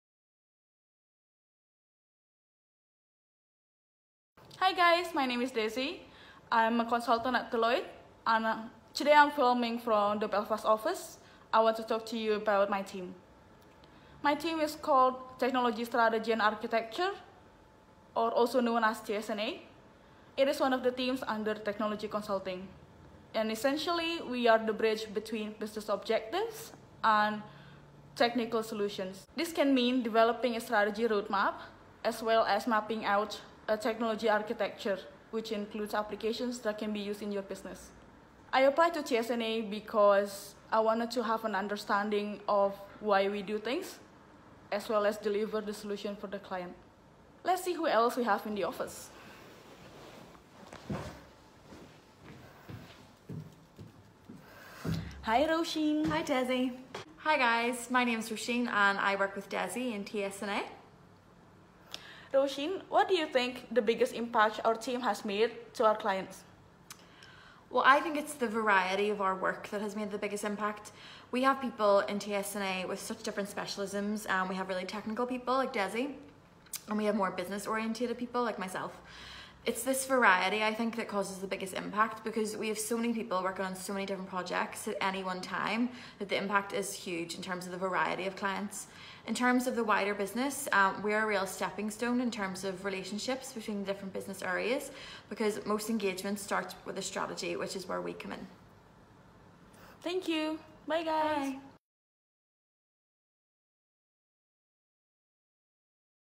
Desy & Roisin: On a Mission
Today, Belfast. Tomorrow, changing the world. Consultant Desy & Analyst Roisin bridge business needs with tech solutions. Here they share what they do, and why. Apply now